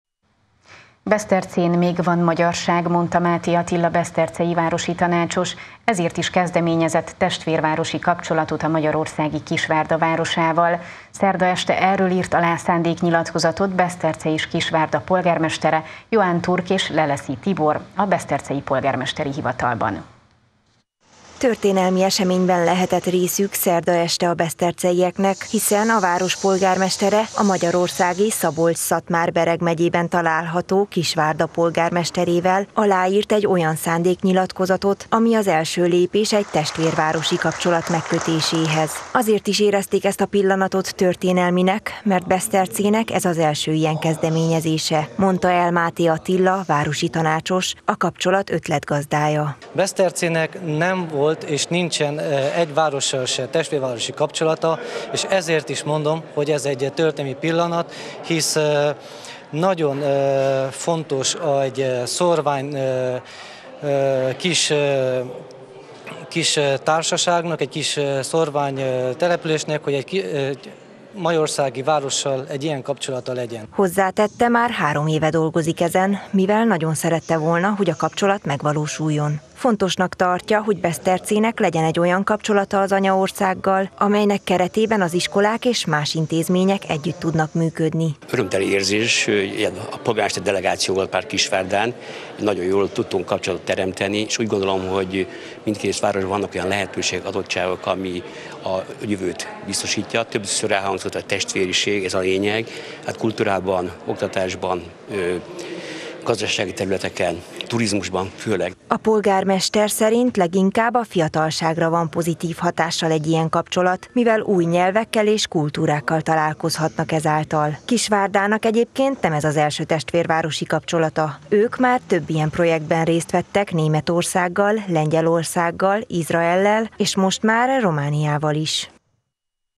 Magyar testvérvárosa lett Besztercének – Erdélyi Magyar Televízió
Besztercén még van magyarság! – mondta Máthé Attila besztercei városi tanácsos. Ezért is kezdeményezett testvérvárosi kapcsolatot a magyarországi Kisvárda városával. Szerda este erről írt alá Beszterce és Kisvárda polgármestere, Ioan Turc és Leleszi Tibor, szándéknyilatkozatot a besztercei polgármesteri hivatalban.